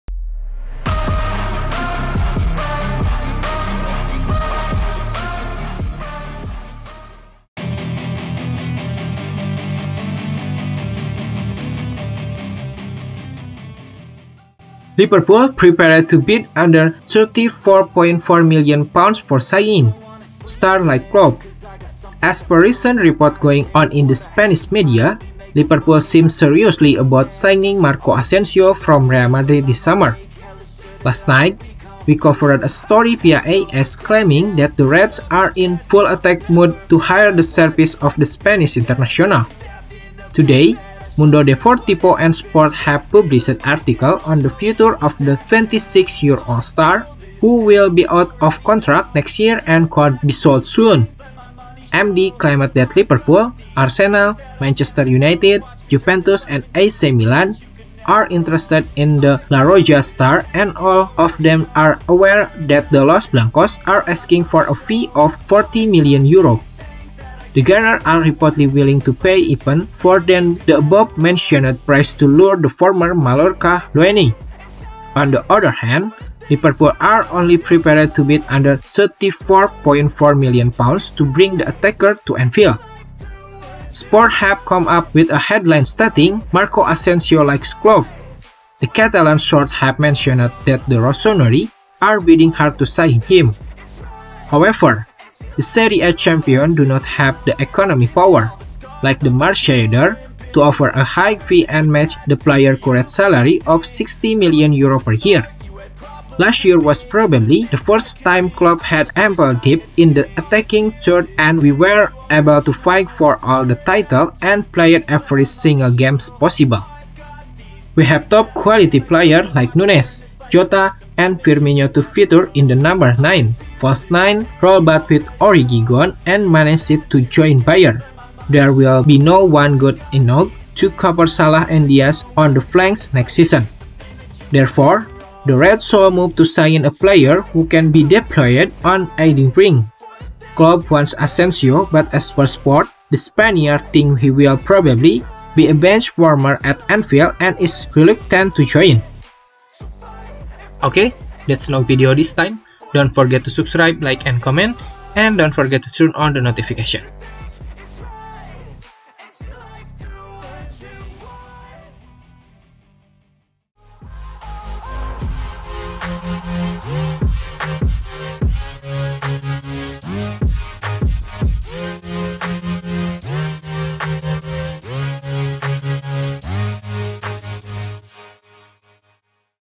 BREAKING NEWS❗STAR LIKES KLOPP❗ Liverpool prepared to bid under £34 4m for signing
As per recent reports going on in the Spanish media, Liverpool seem serious about signing Marco Asensio from Real Madrid this summer.

Last night, we covered a story via AS claiming that the Reds are in full attack mode to hire the services of the Spanish international.

Today, Mundo Deportivo and Sport have published articles on the future of the 26-year-old star, who will be out of contract next year and could be sold soon.